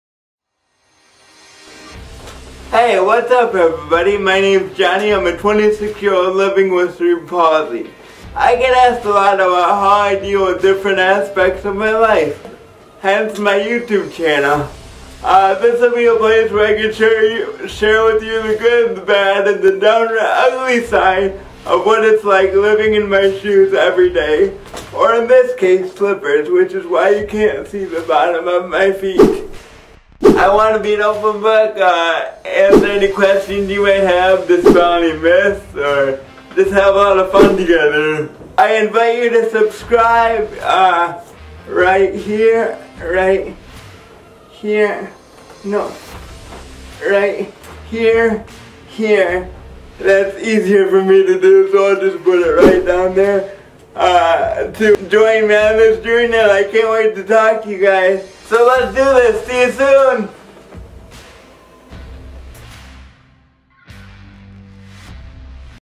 Welcome To My Channel! - Johnny Agar
Hi! My name's Johnny and I have cerebral palsy. I have been very fortunate to have had some pretty cool experiences in my life -being on ESPN, Inside Edition, The Today Show and even in two commercials for Under Armour with Michael Phelps and Dwayne "The Rock" Johnson!

I get asked a lot about how I deal with certain aspects of my disability so I want to invite you into my world to show you what it's like to walk -albeit awkwardly- in my shoes! Each week I will share with you an aspect of my life -the way I deal with setbacks, triumphs and even embarrassing moments, and where I hope to dispel myths or answer any questions you might have!